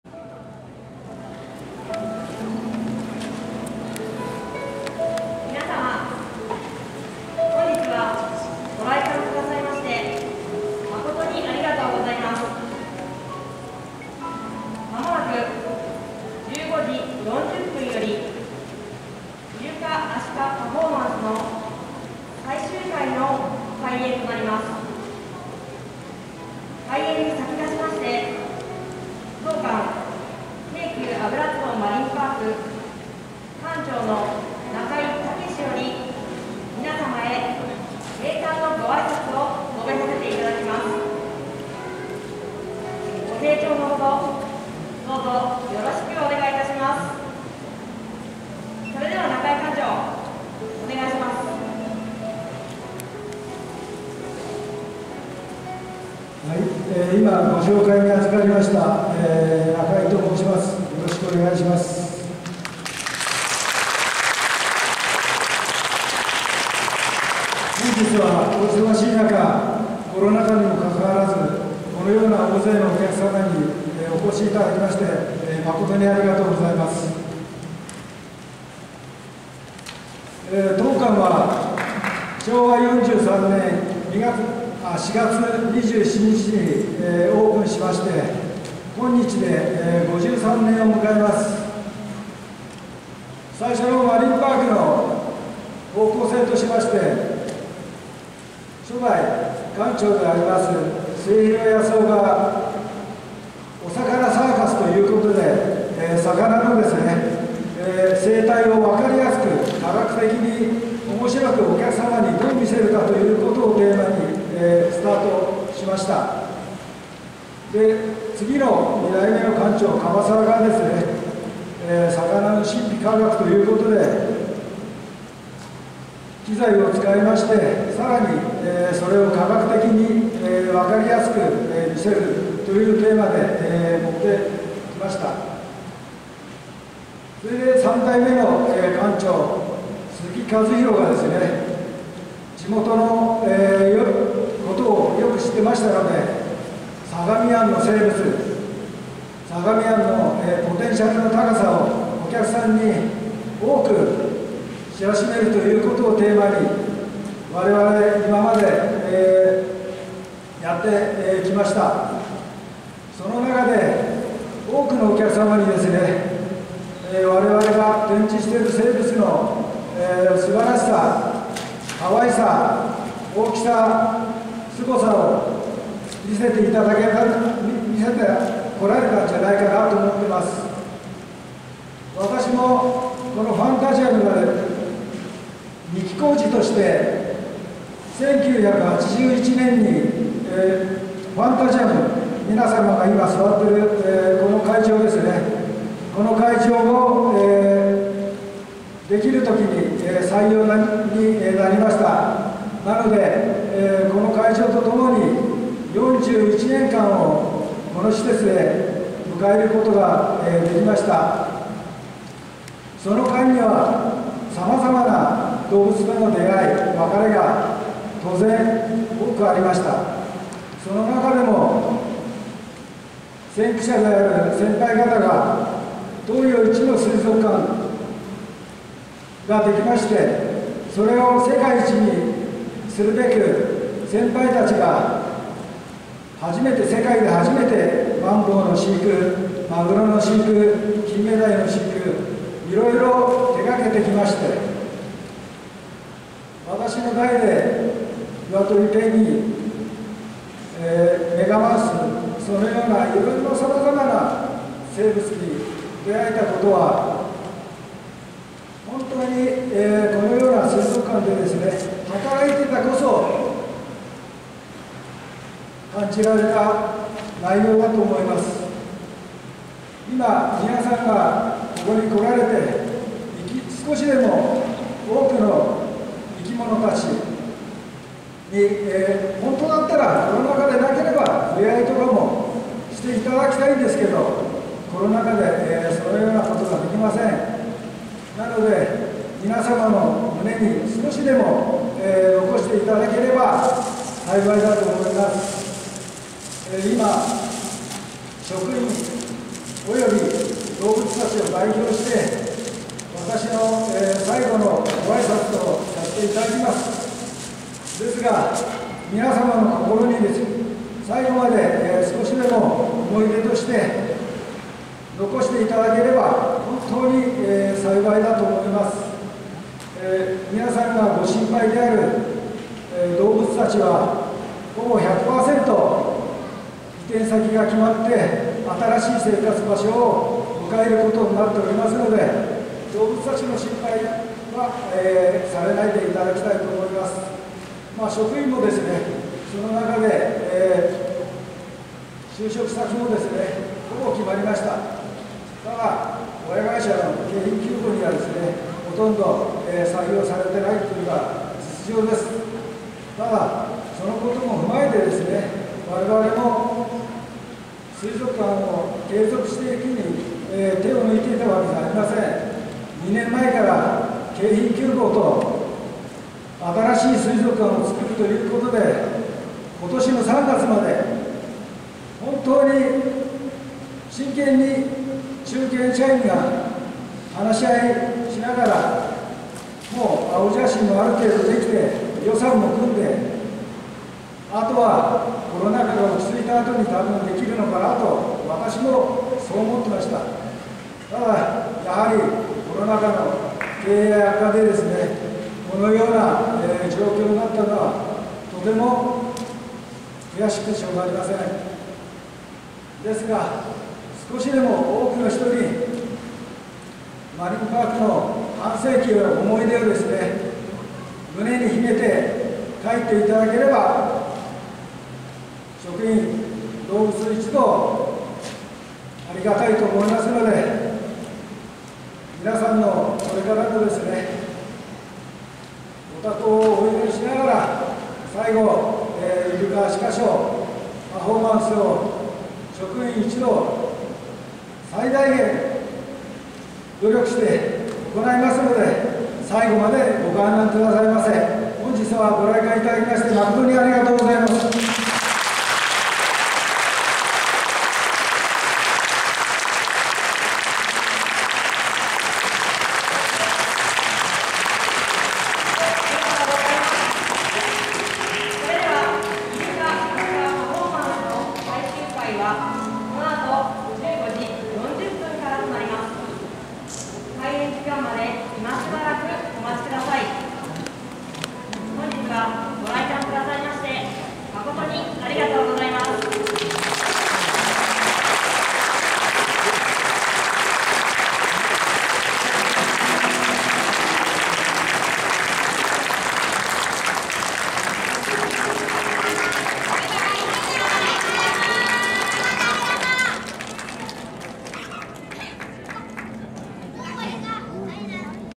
油壺マリンパーク 館長、最後の閉館ご挨拶
2021/9/30で閉館となった油壺マリンパーク 館長によるご挨拶です